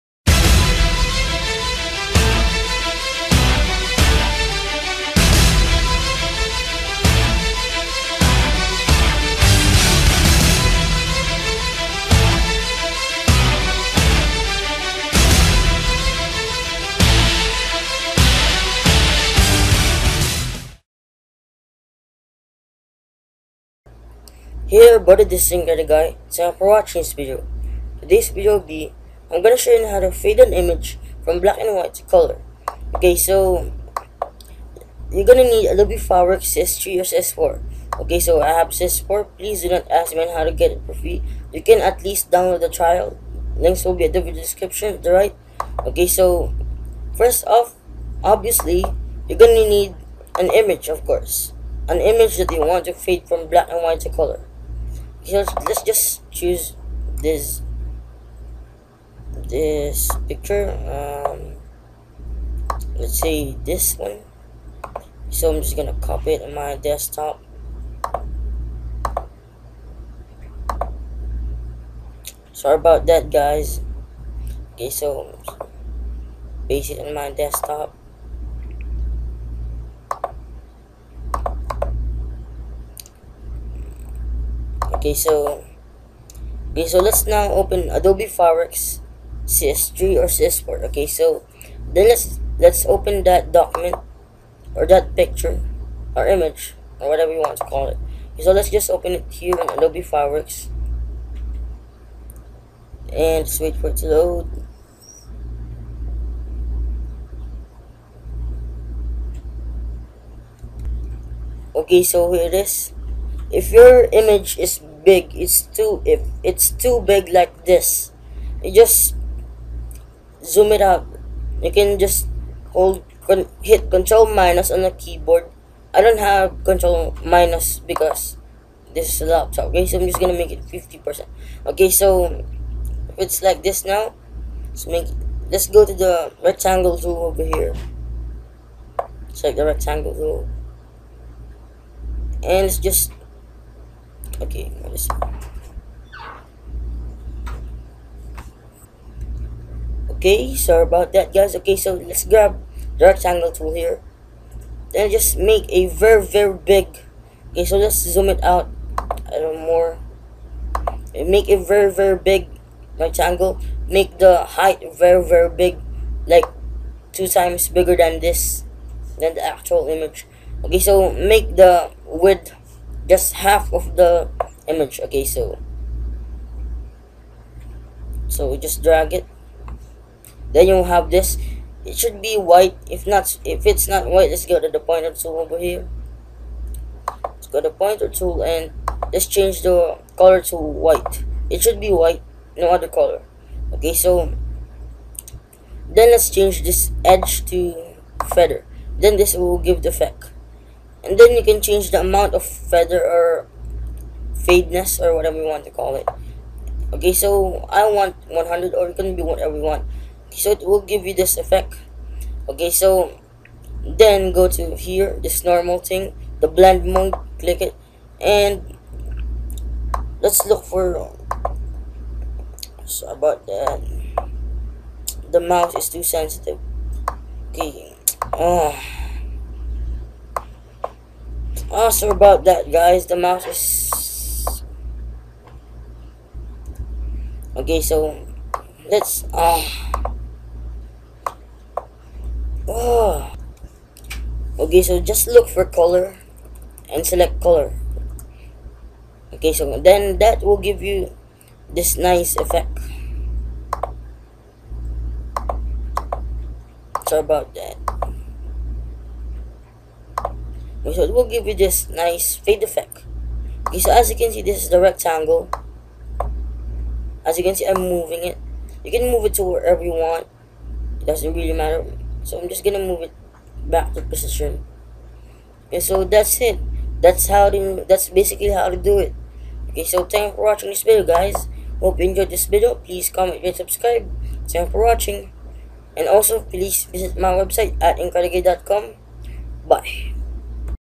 Adobe Fireworks - Fade an Image from Black and White to Color
-teaches you how to fade an image
from black and white to color